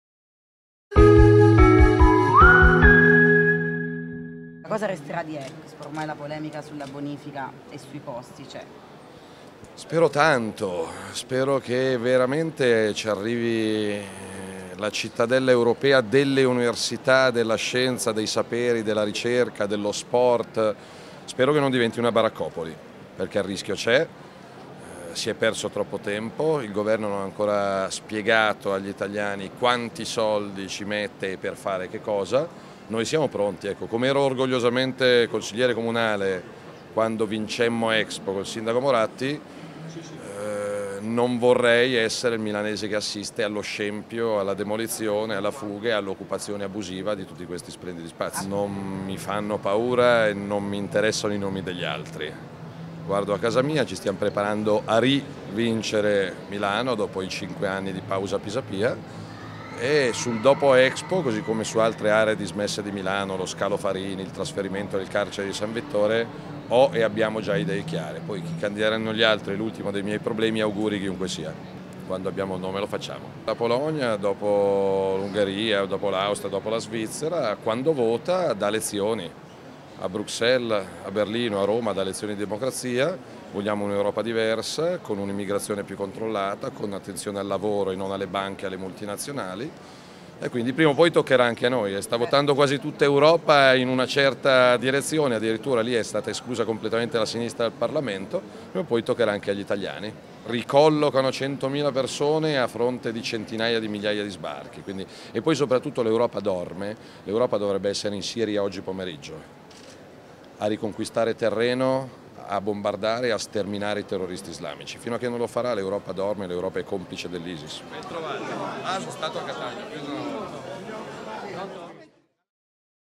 Salvini a Expo: "Il sito non diventi una baraccopoli, a Milano rivinciamo noi"
Mattinata a Expo per il segretario della Lega Nord Matteo Salvini che ha visitato Palazzo Italia e il padiglione della Polonia. Commentando le voci sul dopo Expo, Salvini si è augurato che l'area non diventi "una baraccopoli", mentre sulla Polonia, dove si è votato domenica 25 ottobre con la vittoria della destra anti-UE, Salvini ha detto che "tutta Europa sta andando in quella direzione"